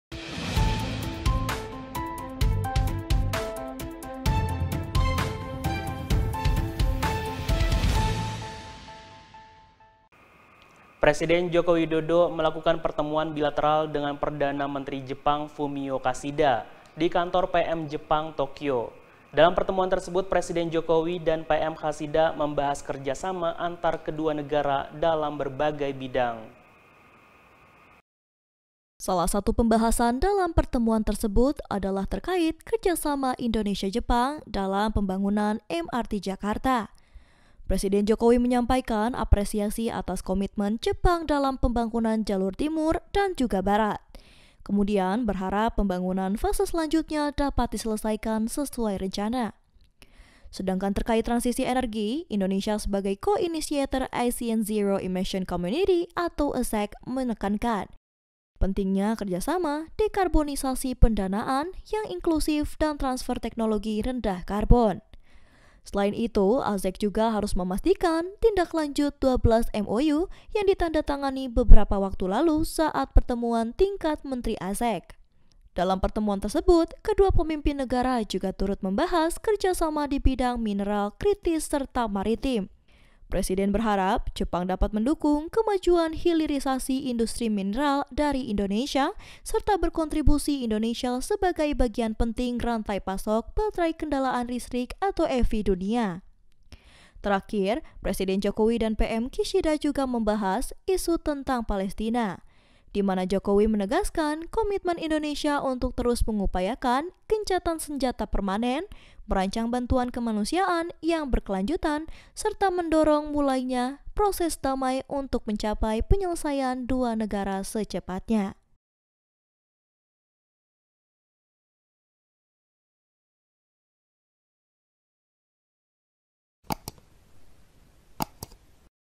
Kerjasama Indonesia dan Jepang Dalam Berbagai Bidang Akan Segera Terealisasikan | UN News | PTV
Presiden Joko Widodo Melakukan Pertemuan Bilateral dengan Perdana Menteri Jepang Fumio Kishida di Kantor PM Jepang, Tokyo. Dalam Pertemuan Tersebut, Presiden Jokowi dan PM Kishida Membahas Kerja Sama Antar Kedua Negara dalam Berbagai Bidang.

Simak Video Berikut untuk Berita Selengkapnya ya, Hanya di UN News PTV.